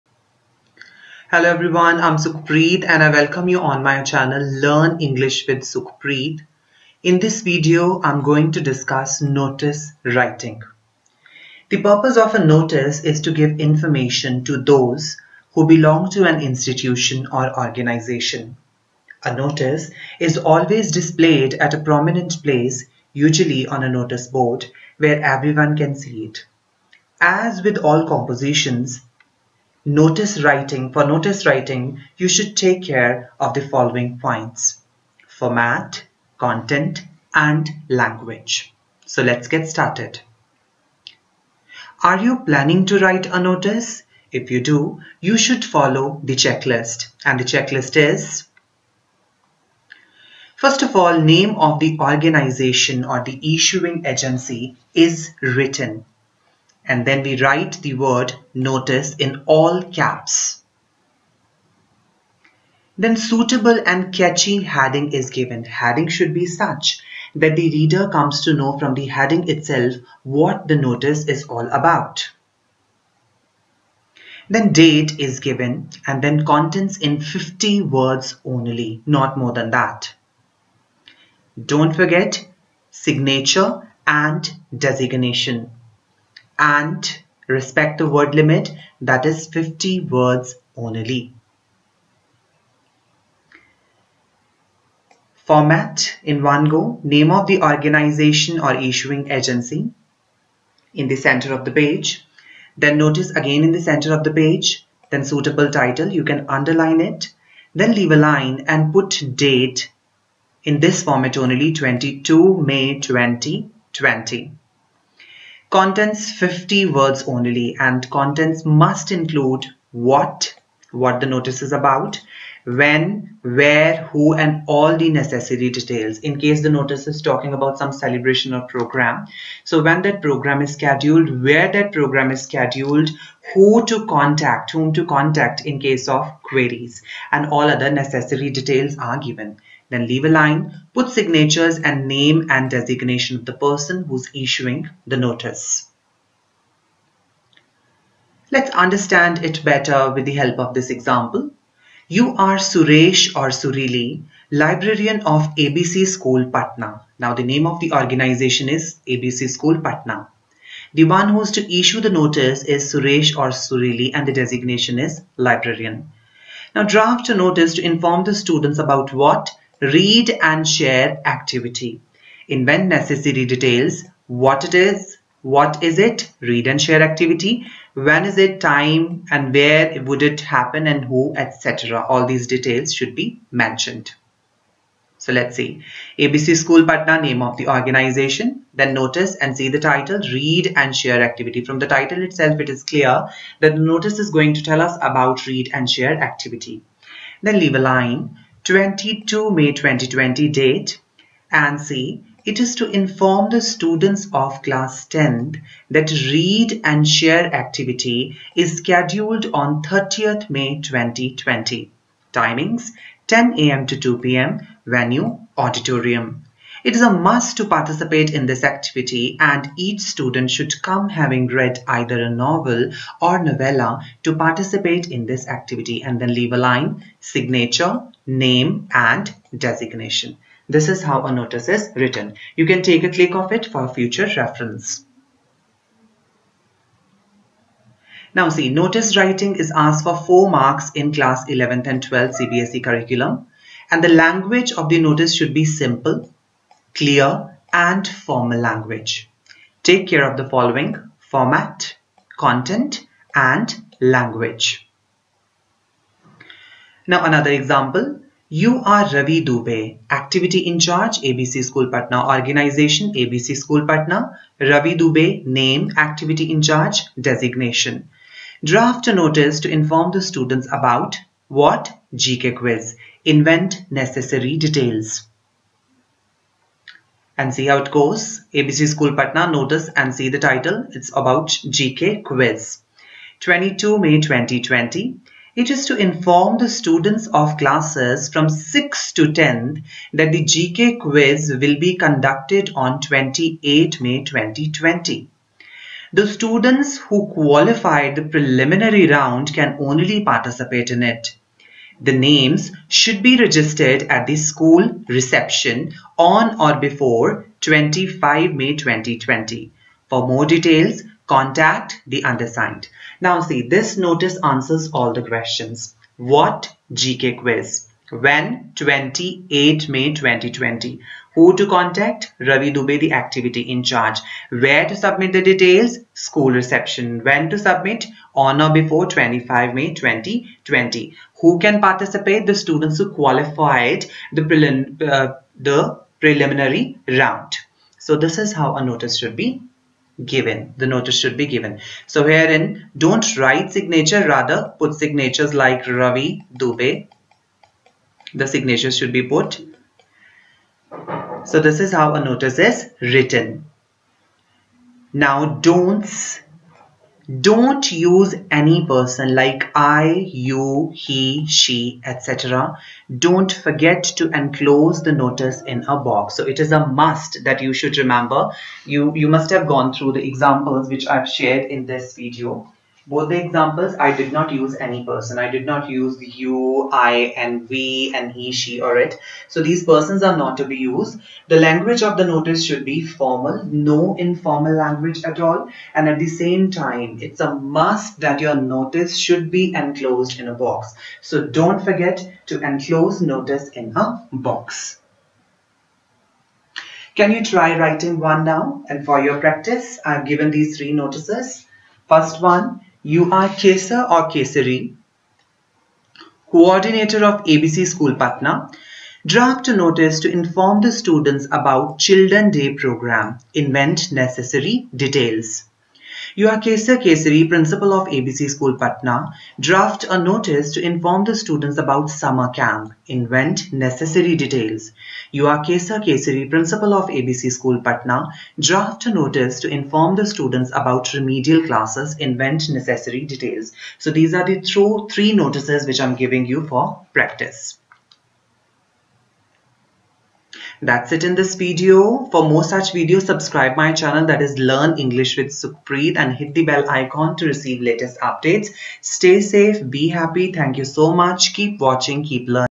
Notice Writing I Class VIII to XII CBSE I
The video discusses the format, content and language for drafting a notice. Notice writing is in the syllabus of Grade XI & XII CBSE and a question is asked on this for 4 marks.